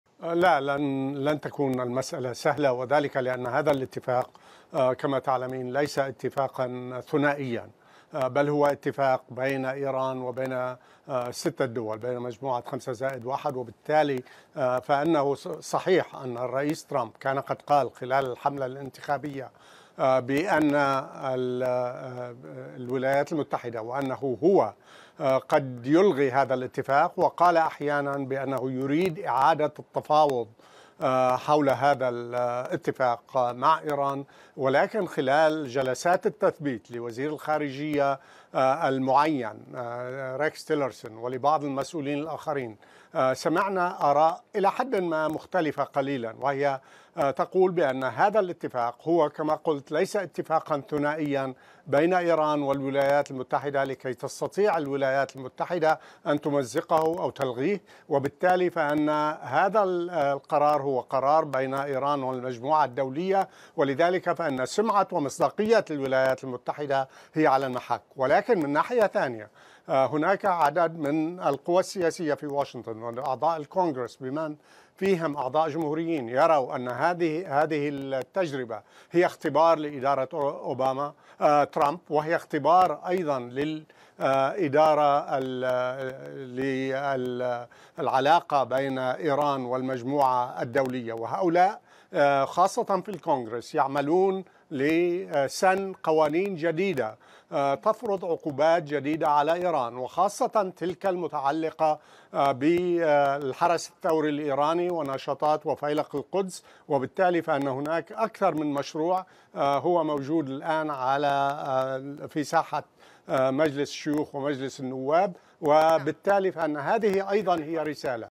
خبير في الشؤون الأمريكية: لهذه الأسباب لن يستطيع ترامب إلغاء الاتفاق النووي
عدَدَ إدموند غريب، الخبير في الشؤون الدولية والأمريكية، مجموعة من الأسباب التي تقف ضد رغبة ترامب في إلغاء الاتفاق النووي مع إيران، كما صرح خلال حملته الانتخابية.